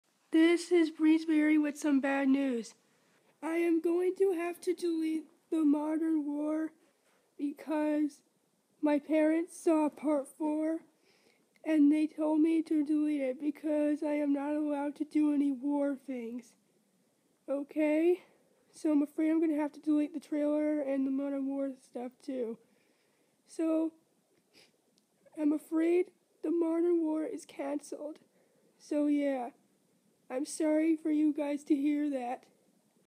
Bad News!
I'm going to have to cancel The Modern War. Why? Because when my parents saw it, they told me to delete it because they told me to not do any War thing.